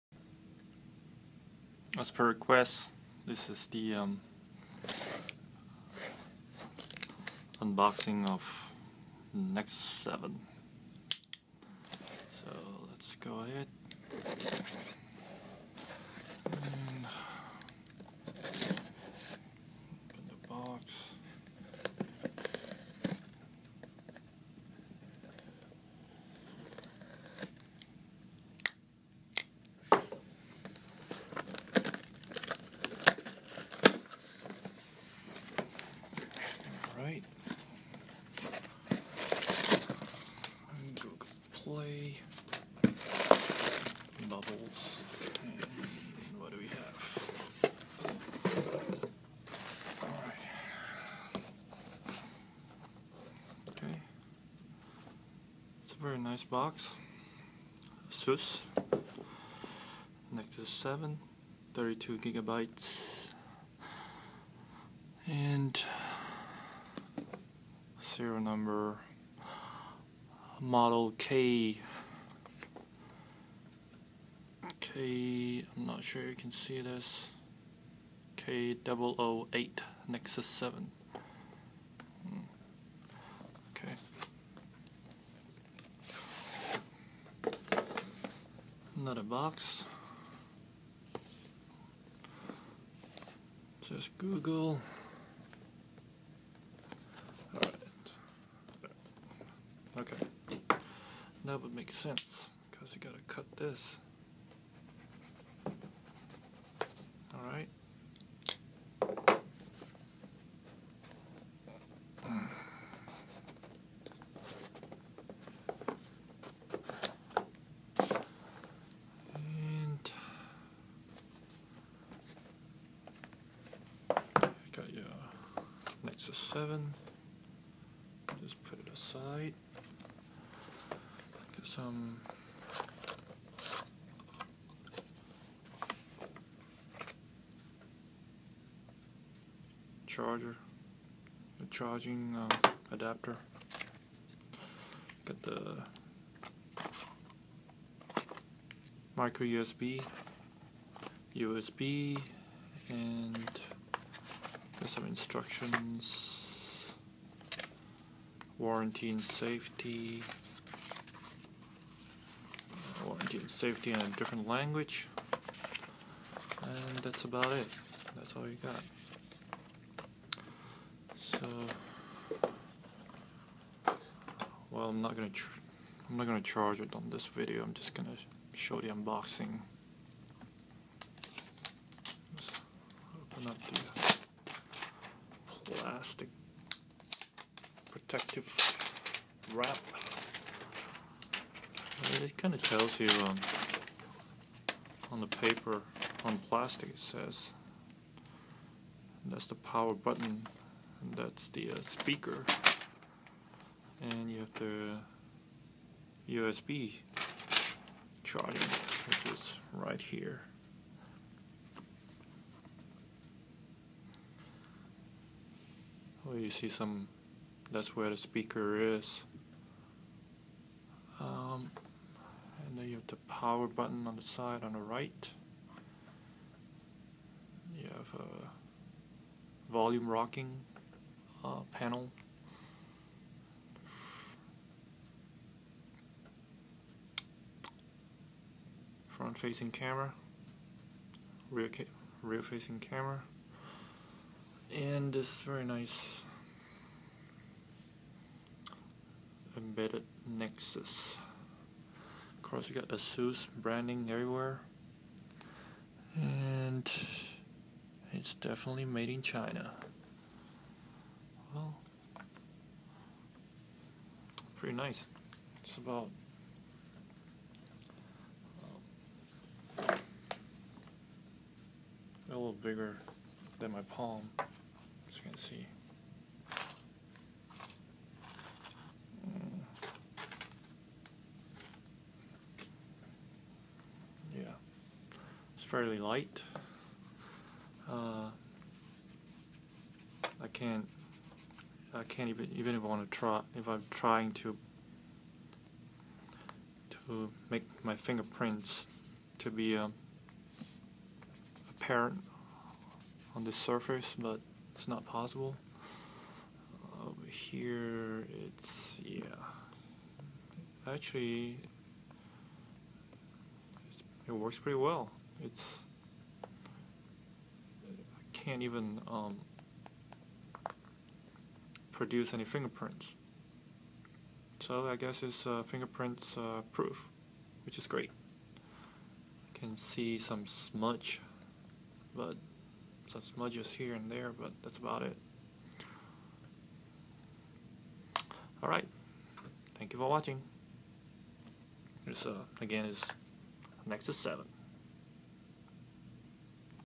2013 Google Nexus 7 32GB quick unboxing (no specs)
Quick unboxing of 2013 Google Nexus 7 32GB edition. For technical specifications, please Google Nexus 7 :p